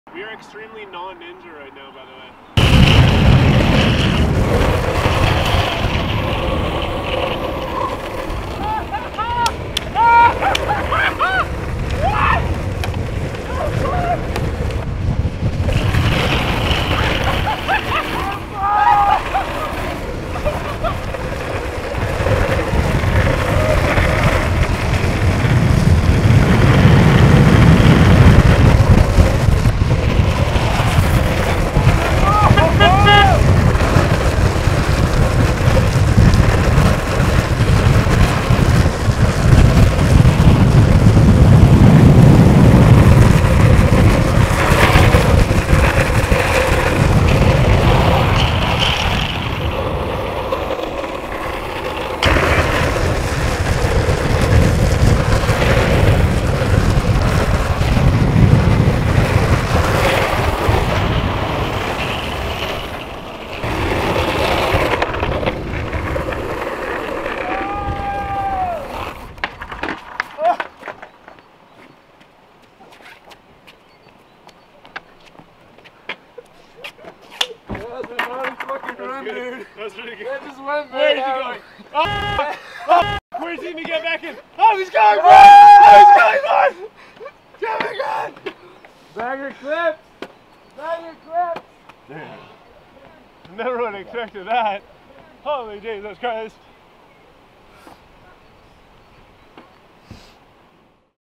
Patrick and Deen - Extremely Non Ninja
Deen and Patrick slicing and dicing on the Darkside for the Sicktrip 2018.